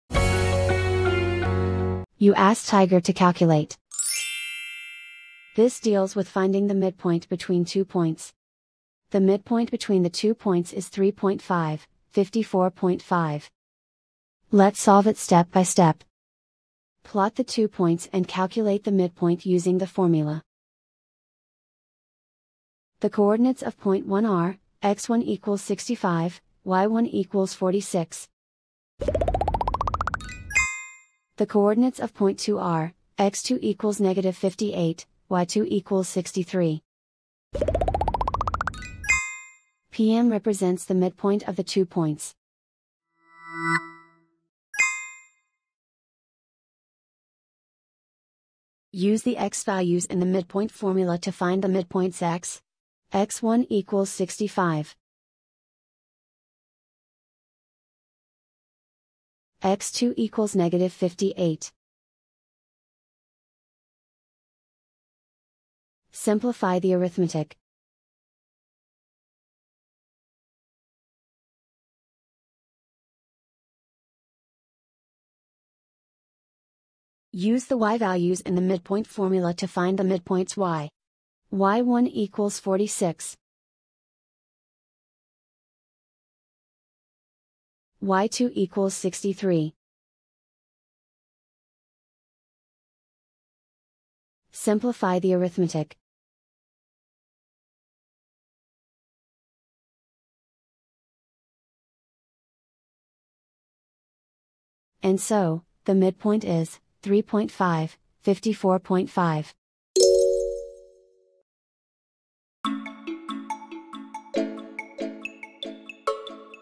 Find the midpoint between two points p1 (65,46) and p2 (-58,63): Step-by-Step Video Solution
See how to easily find the midpoint between two points p1 (65,46) and p2 (-58,63) with our clear, step-by-step video guide on Tiger Algebra. Also, check out for more tips and tricks